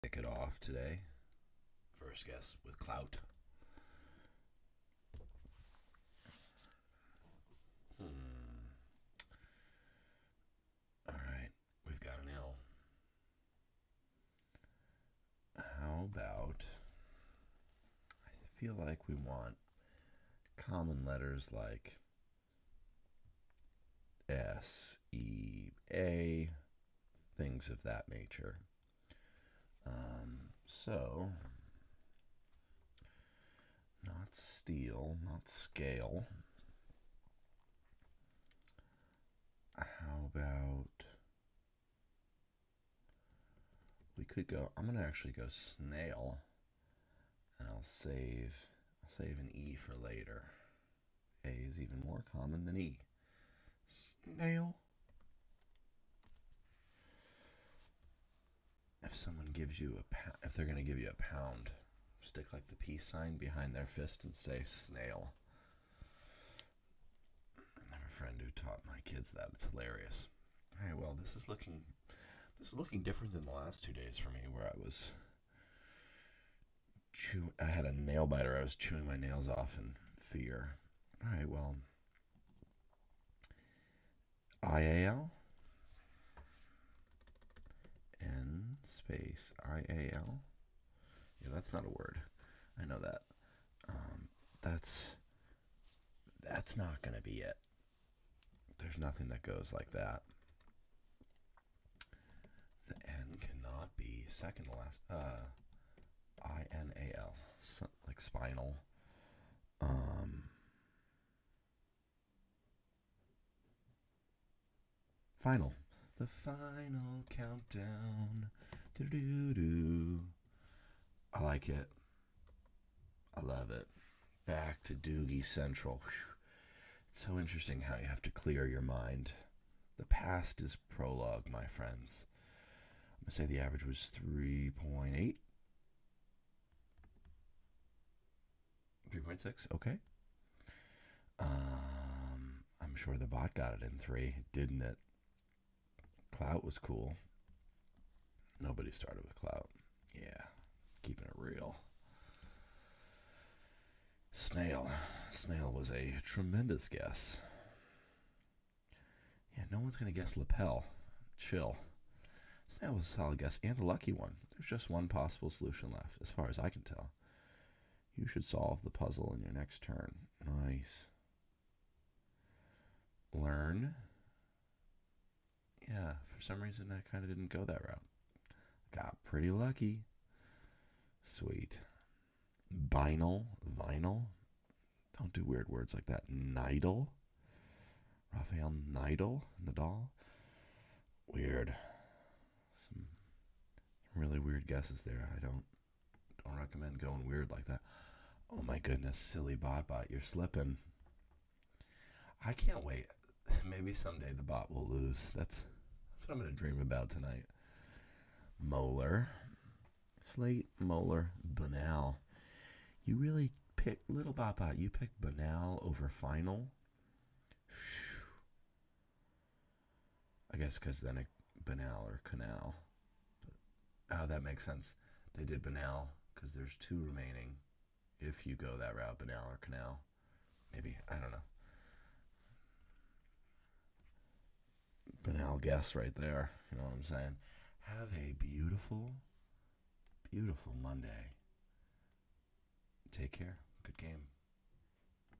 Today's Daily Wordle Answer
Hi Team 👋! Watch me answer today's Wordle. I go through my thought process and crush it!

Watch me Dougie today's Wordle. What does that mean? Well, Neil Patrick Harris claims he "always" gets the Wordle in three (yeah right!).